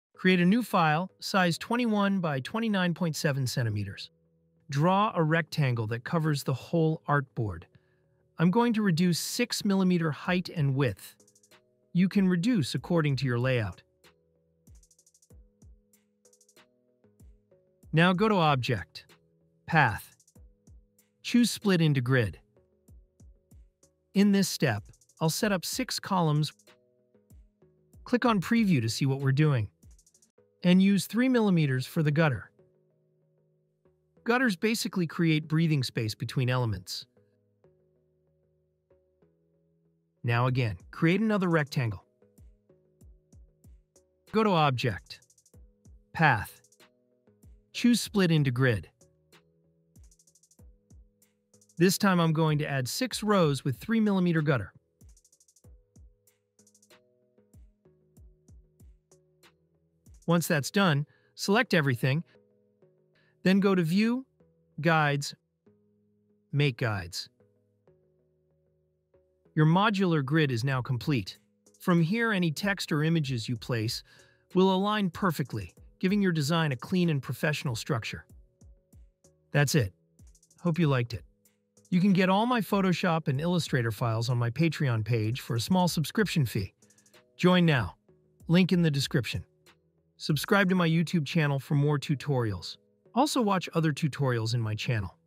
Design Faster with Grids in Illustrator !!
► DESIGN TOOLS I USE

In this tutorial, I’ll show you how to create professional magazine grids in Adobe Illustrator — perfect for both beginners and pros.